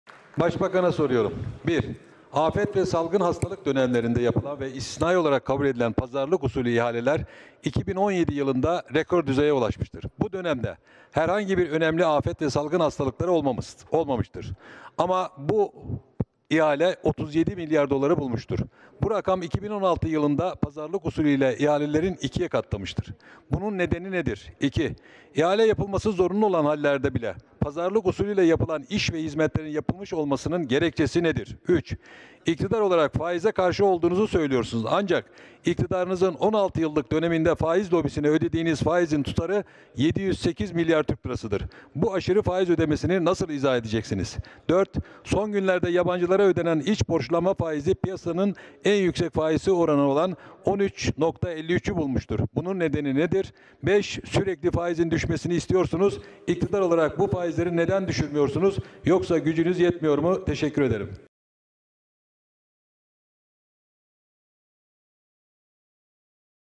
Yaptığınız İhalelerin Gerekçesi Nedir?
Denizli Milletvekilimiz Kazım Arslan:  Afet ve salgın hastalık dönemlerinde yapılan ve istisnai olarak kabul edilen pazarlık usulü ihaleler 2017 yılında rekor düzeye ulaşmıştır. Pazarlık usulüyle ihalelerin 2'ye katlanmasının sebebi nedir?